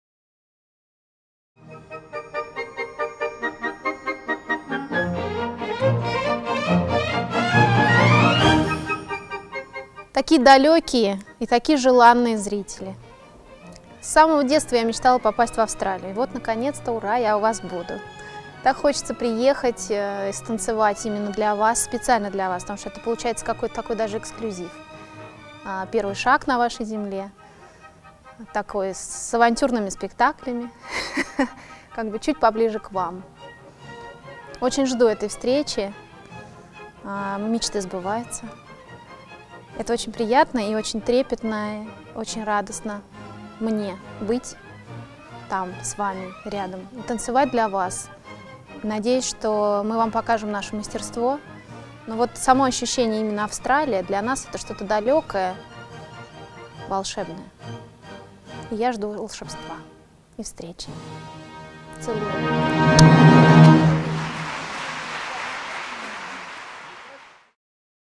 A Greeting from The Bolshoi Ballet
The lovely Maria Alexandrova, principal dancer of the Bolshoi Ballet, sends a message about Bolshoi's upcoming visit!